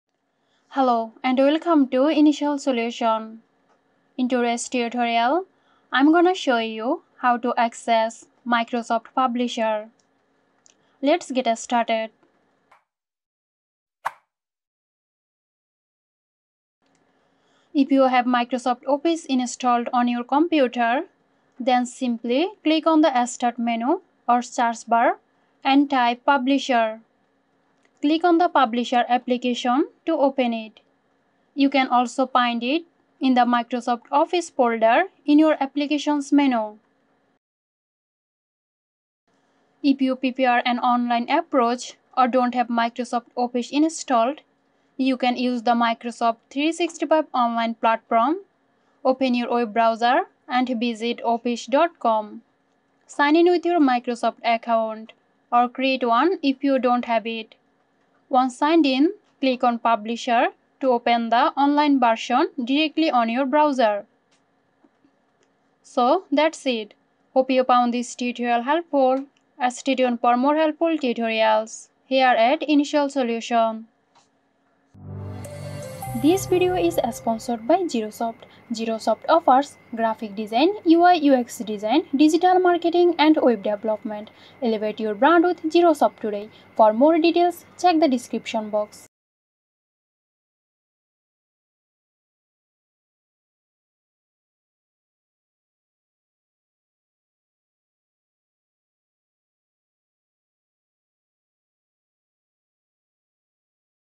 🌐 How to Access Microsoft Publisher: Quick Guide | 2024
Get ready to unleash your creativity! In this quick guide, we'll walk you through the steps to access Microsoft Publisher, a powerful tool for designing publications, marketing materials, and more.

📌 Step-by-Step Guide:

1. Via Microsoft 365 (Online):
Open your web browser and go to the Microsoft 365 website.
Sign in with your Microsoft account.
Click on the "Apps" menu and select "Publisher."

2. On Windows:
If you have Microsoft Office installed, you may find Publisher in the Microsoft Office suite.
Open the Start menu, scroll through your applications, and find Microsoft Publisher.

3. Accessing Publisher Templates:
Once in Publisher, explore the available templates to kickstart your project.

4. Start a Blank Document:
If you prefer, start with a blank document and unleash your creativity from scratch.

5. Save and Export Your Project:
Save your work as you go, and when you're finished, export or print your publication.
Alternative Method: Using Publisher Online:
If you don't have the desktop version, you can use the online version of Microsoft Publisher available on the web.